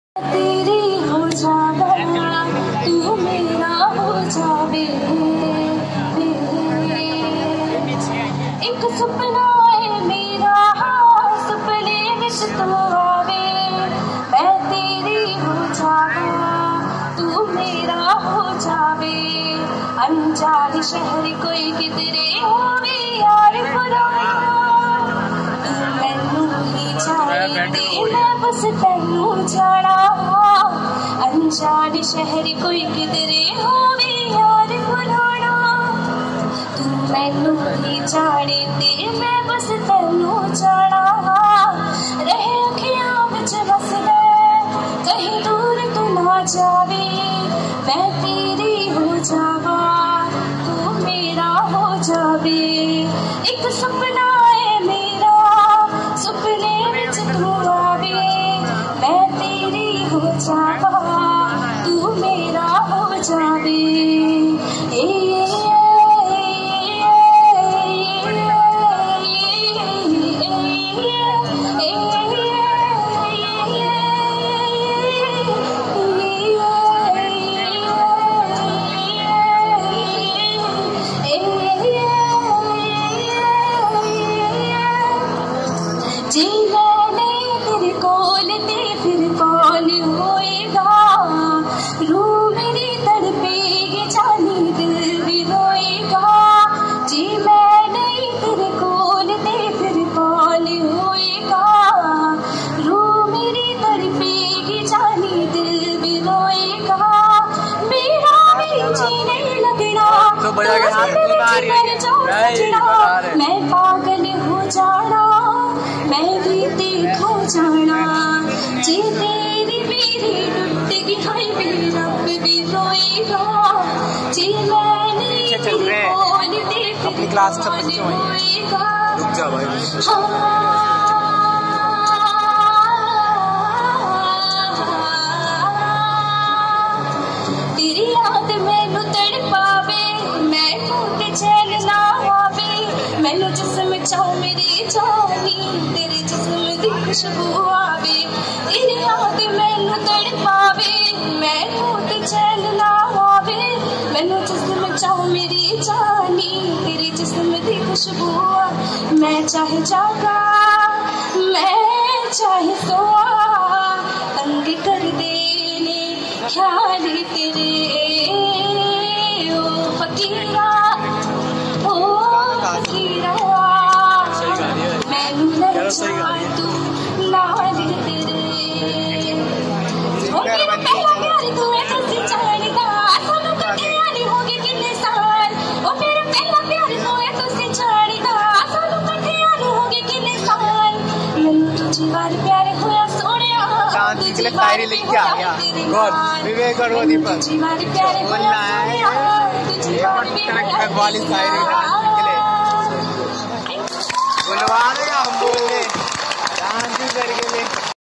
farewell party video | Beautiful songs cover by girls | Government model senior secondary school
Farewell party video | Beautiful songs cover by girls with beautiful voice | Government model senior secondary school | faridabad |sarai khawaja |